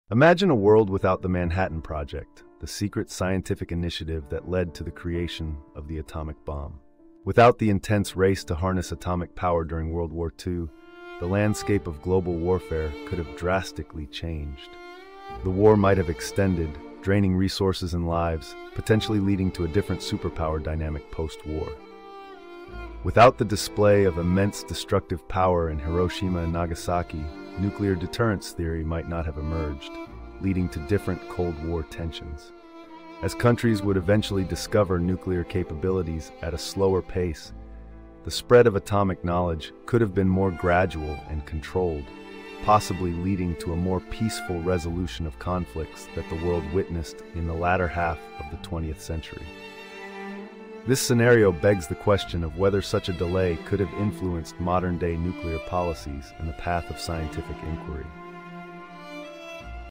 What If The Manhattan Project Never Existed?
Explore the alternative history where the Manhattan Project was never realized and its potential global impact.